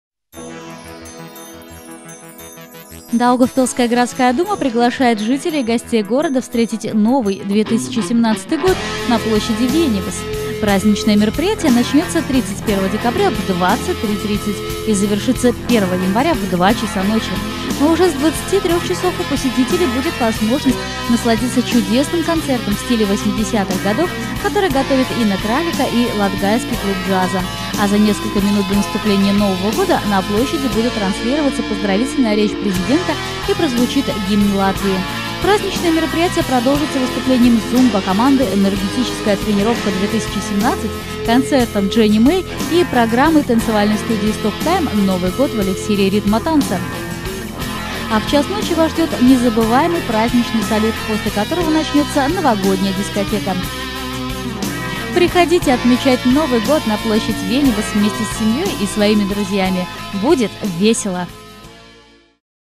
Вcтретим Новый 2017 год на площади Виенибас в Даугавпилсе!
Даугавпилсская городская дума поздравляет жителей и гостей города с наступающим Новым годом и приглашает встретить его вместе на площади Виенибас.